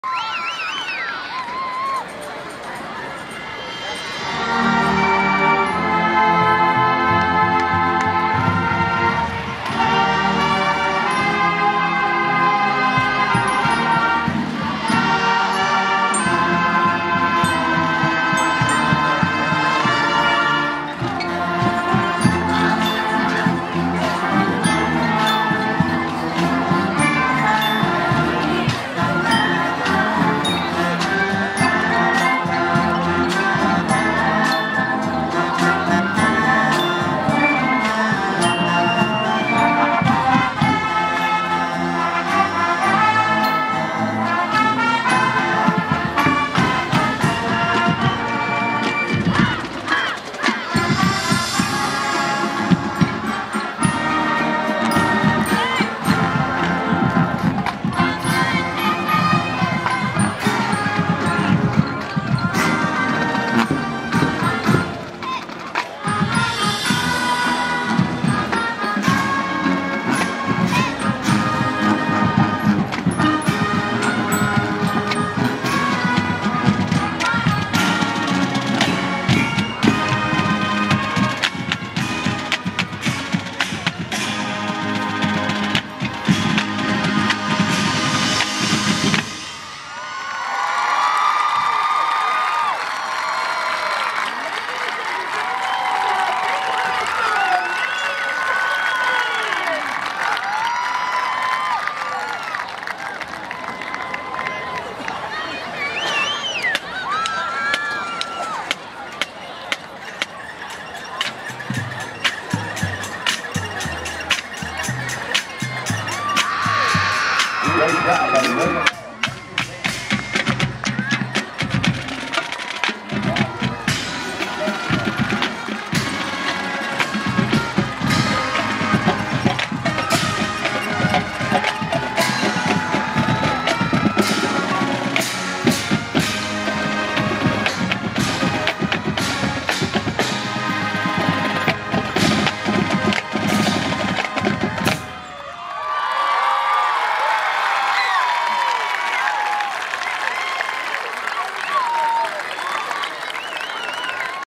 2011 Bay vs Arnold Halftime Show pt 2
taped from the end zone

Drum Major: Kayla Boston
Band Captain: Allen Parrish
Director of Bands: Nicholas Efstathiou
Percussion Instructor: Mark Mitchell
Jonathan Soyka , Color Guard Director| Bay Breeze Winterguard Instructor
Megan Cash, Asst. Color Guard Director |Bay Breeze Winterguard Instructor
Christopher Dean -- Million Dollar Choreographer, Dance Director, Bay Breeze Winterguard Staff
Kelly S. Efstathiou, Auxiliary Director|Majorette Instructor